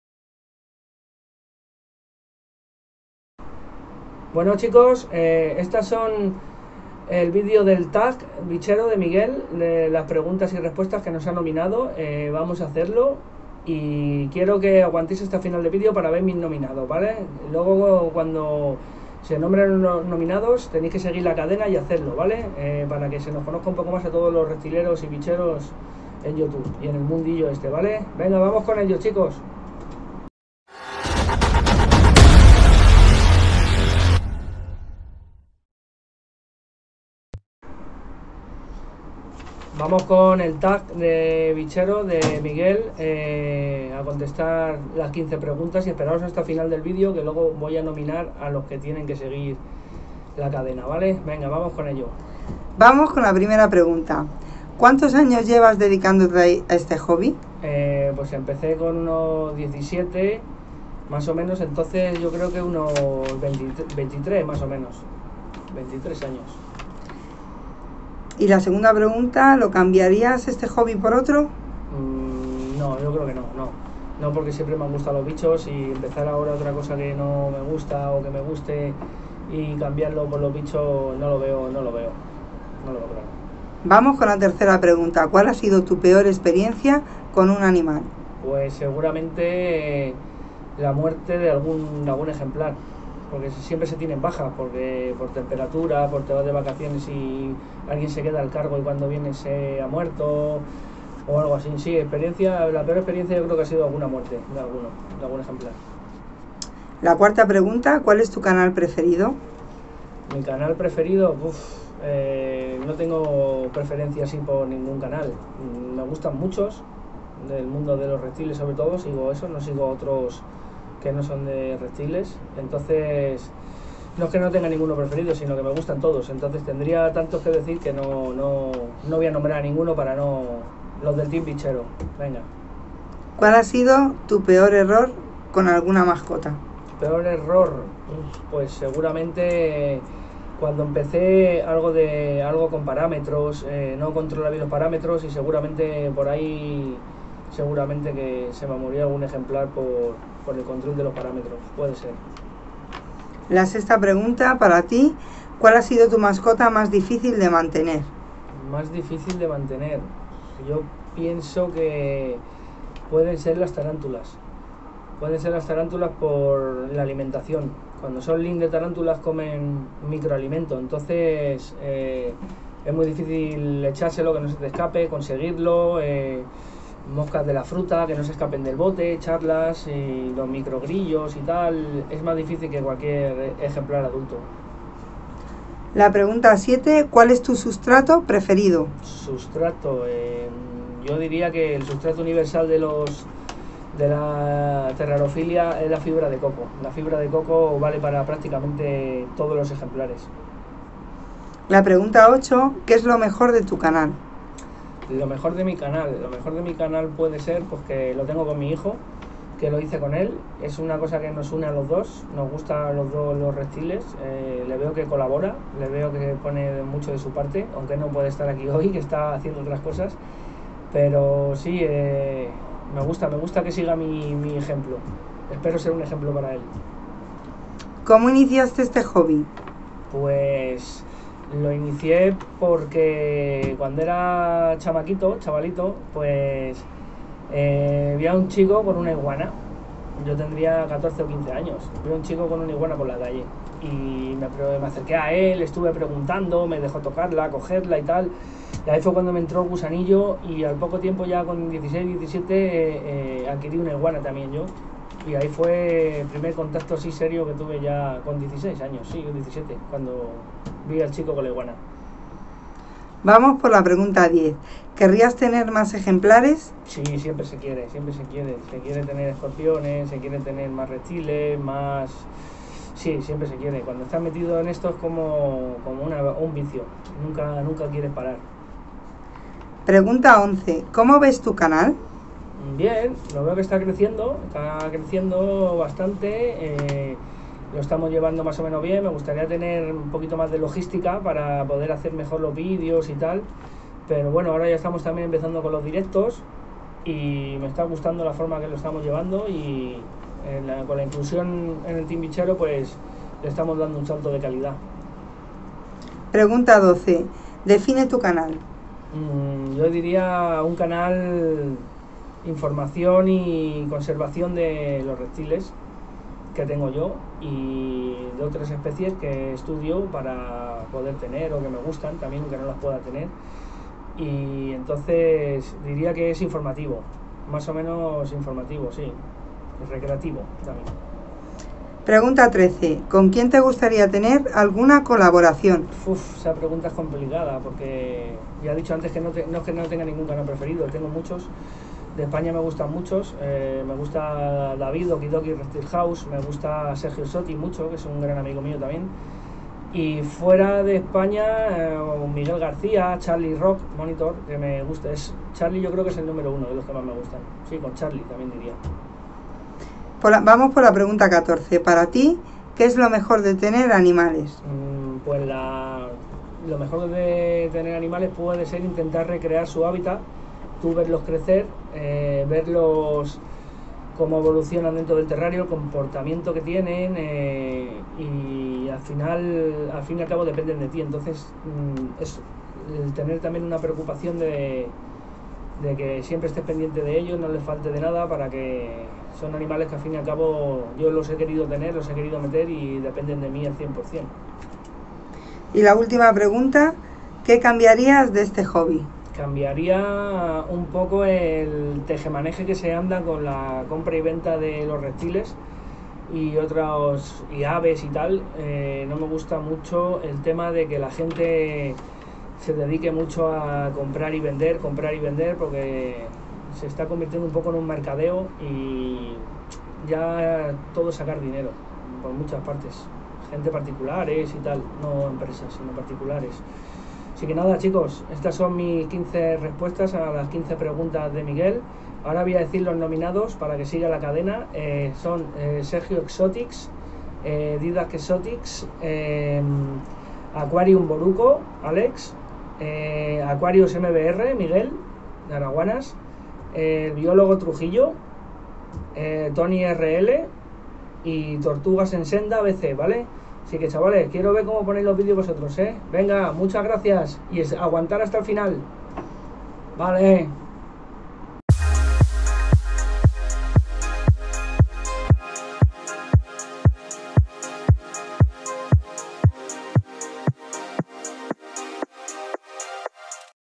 TAG BICHERO
Hemos hecho el reto de Miguel Bio Exotics de las 15 preguntas y 15 respuestas.
Estos son los nominados por mi:
Que la cadena no pare, seguir con las nominaciones.